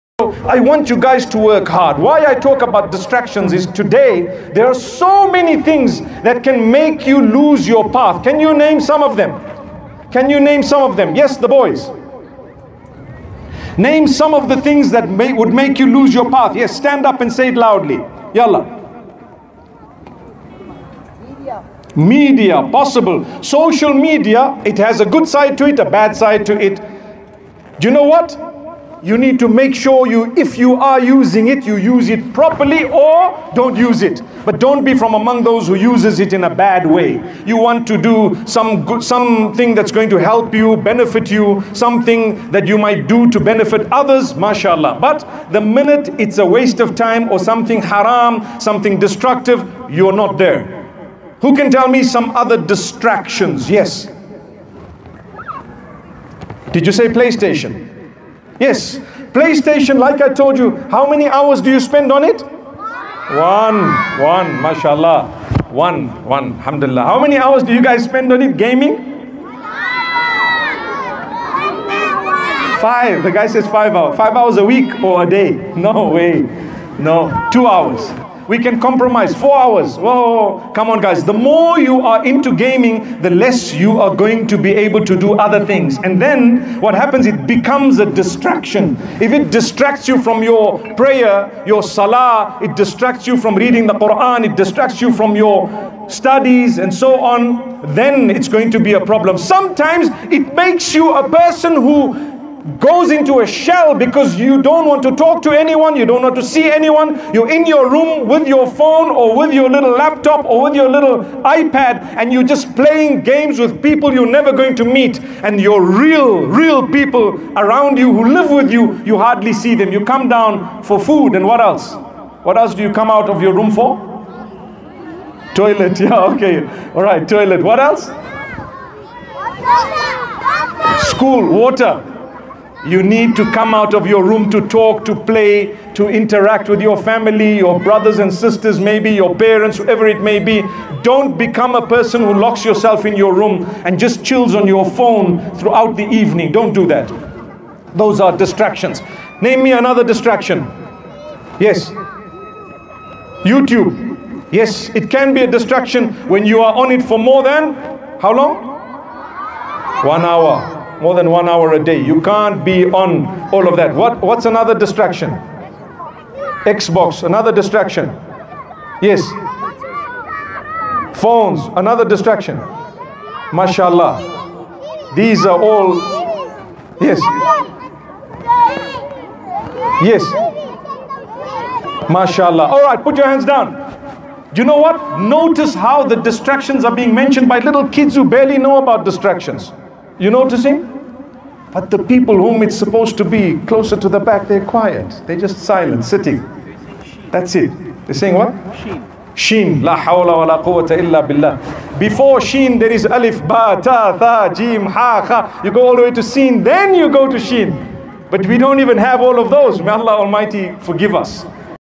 Don't Allow Distractions to Hijack Your Life: MUFTI MENK
Mufti Menk emphasizes the importance of maintaining focus and avoiding distractions that can corrupt one's life. He discusses how distractions, particularly in our modern world, can lead us away from our spiritual goals and the remembrance of Allah. Mufti Menk urges listeners to be vigilant about what consumes their time and energy, reminding them that life's ultimate purpose is to worship Allah and live in accordance with Islamic principles.